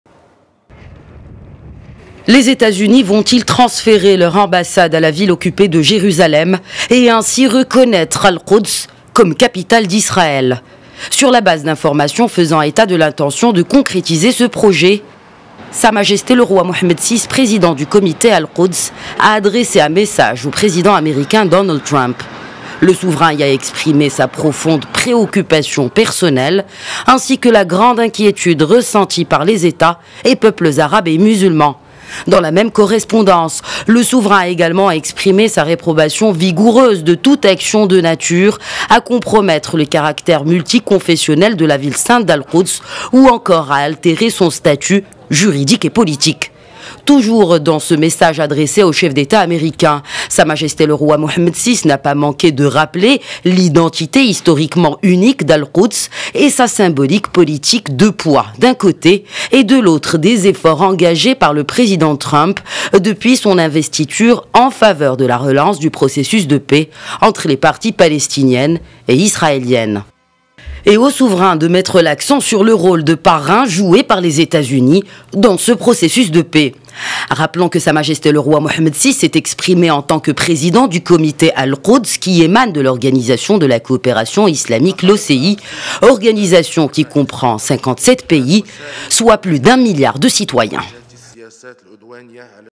Al-Qods: message du roi Mohammed VI au président Trump
Le Roi Mohammed VI, président du Comité Al-Qods, a adressé, mardi, un message au président des États-Unis. Le Souverain fait part à Trump de sa profonde préoccupation suite aux informations récurrentes sur l'intention de l'administration américaine de reconnaître Al-Qods comme capitale d’Israël.